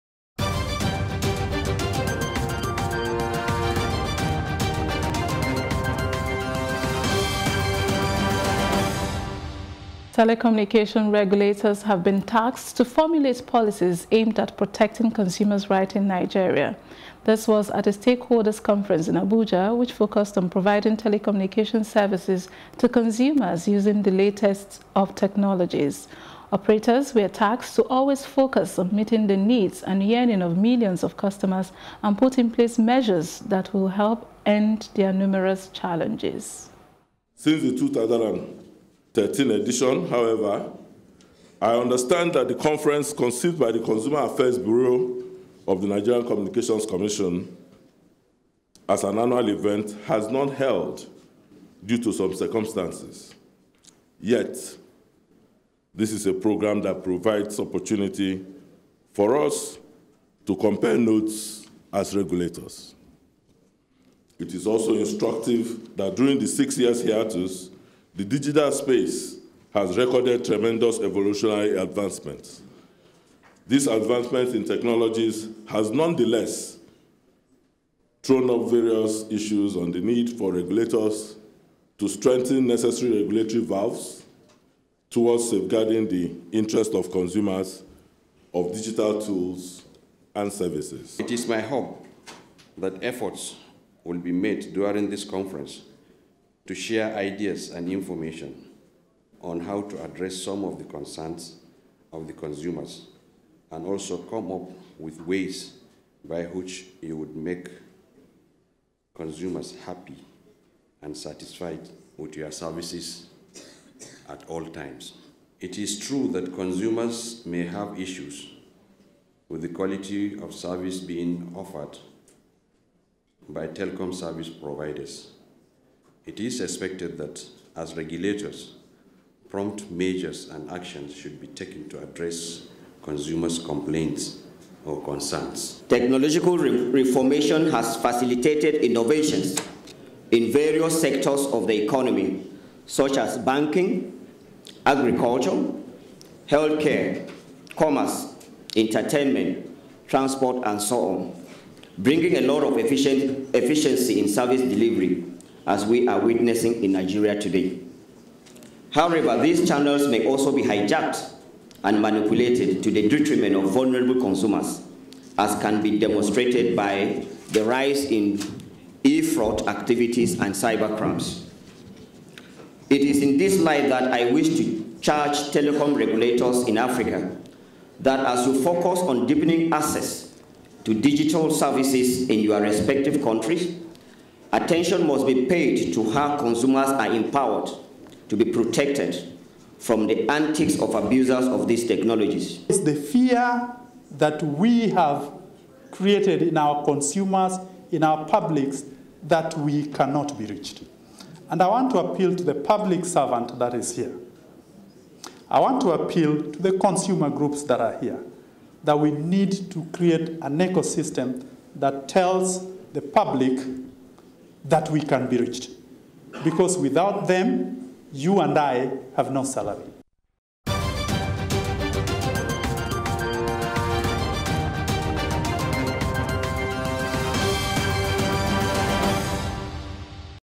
Why Telecoms Operators Must Treat their Consumers as Kings
Telecommunication regulators have been tasked to formulate policies aimed at protecting consumer's right in Nigeria.
This was at a stakeholders’ conference in Abuja which focused on providing telecommunication services to consumers using the latest of technologies.